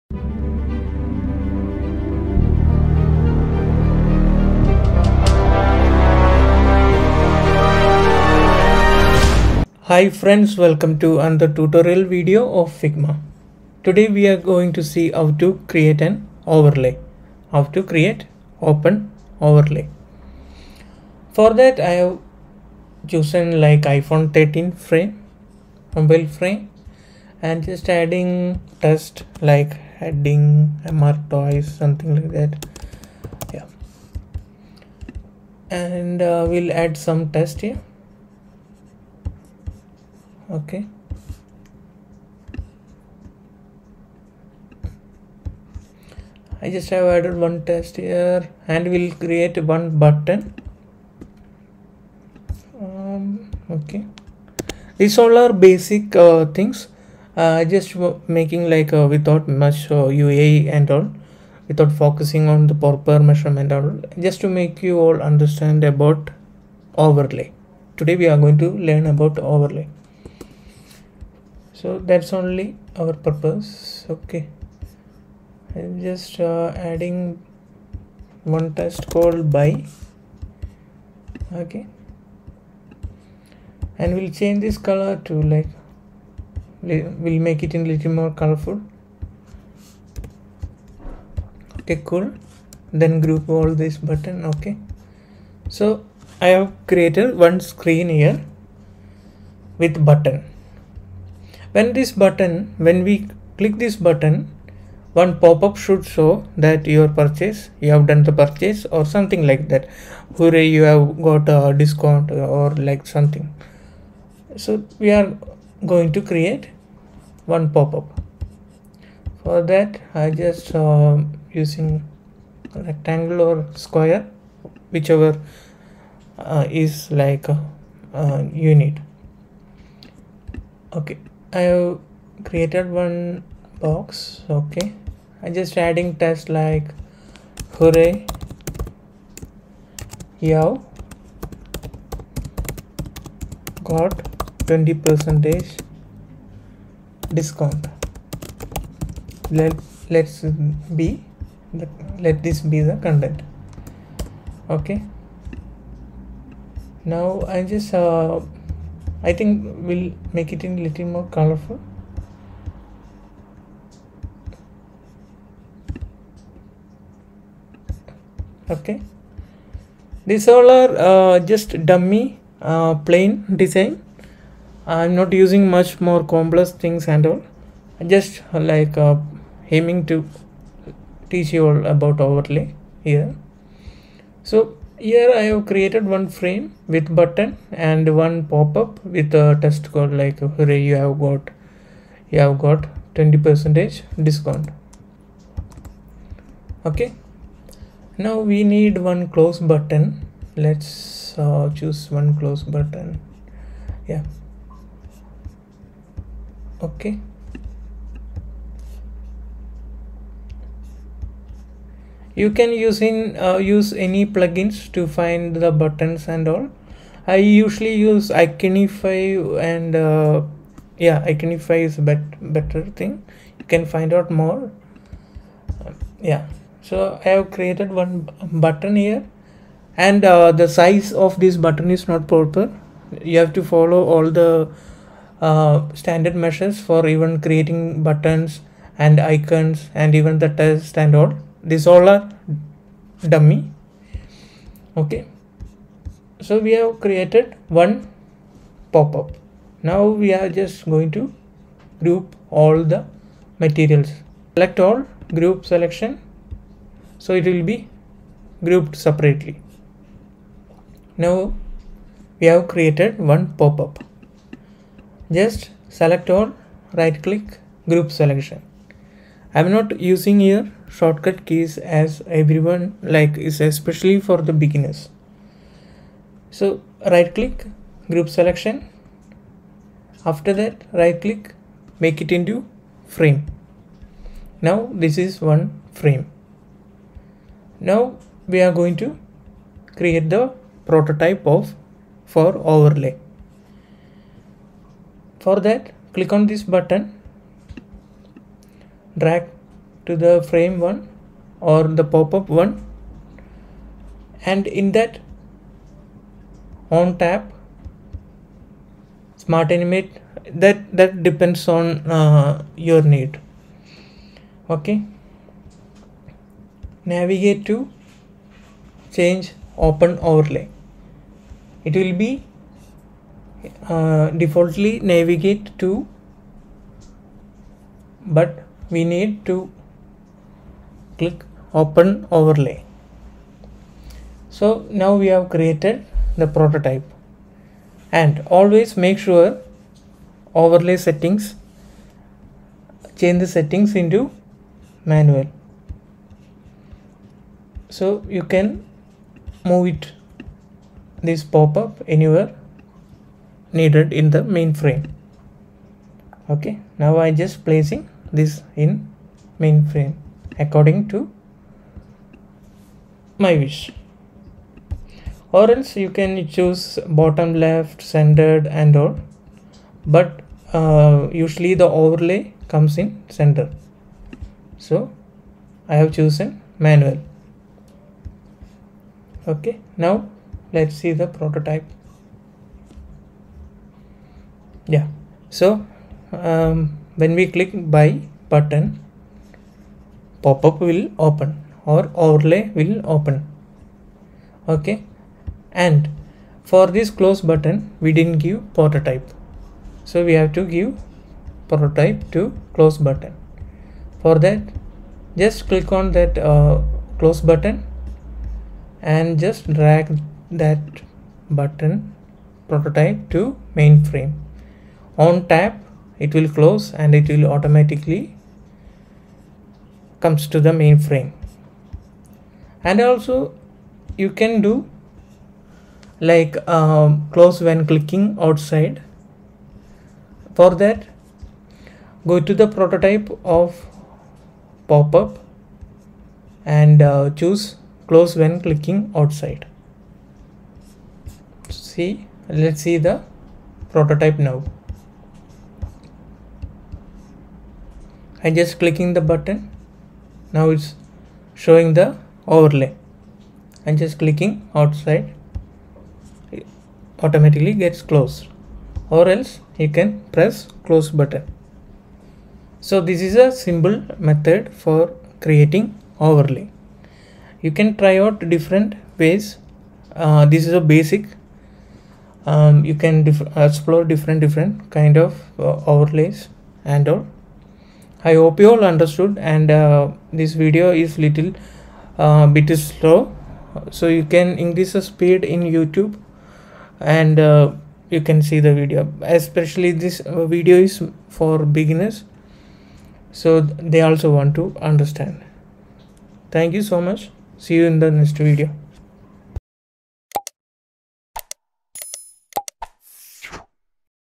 Mastering Figma: Create Stunning Overlay Prototypes | Figma Tutorial
Welcome to our Figma Tutorial on creating sleek overlay prototypes! In this step-by-step guide, you'll gain the skills to design stunning UI/UX overlays. Elevate your design game today!

Follow Jishnucreation2021 Instagram